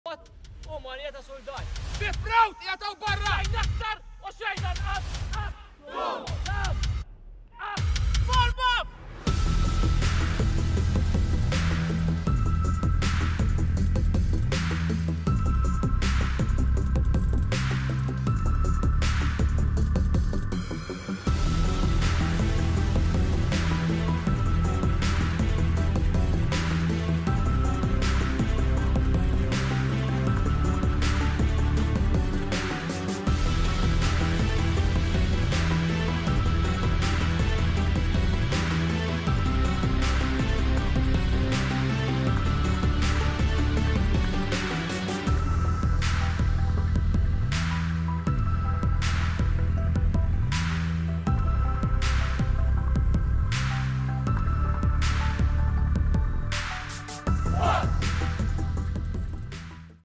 Recruitment Intake 221 - Week 2
🎥 Here’s a look at Week 2 of training for Recruits forming part of Recruitment Intake 221! 🇲🇹💂🏻‍♀️

This week the instructors focused on Military Drill, Combat Physical Training and introduction to weapons. Recruits also underwent a number of inspections on their uniform turn-out and common areas.

See less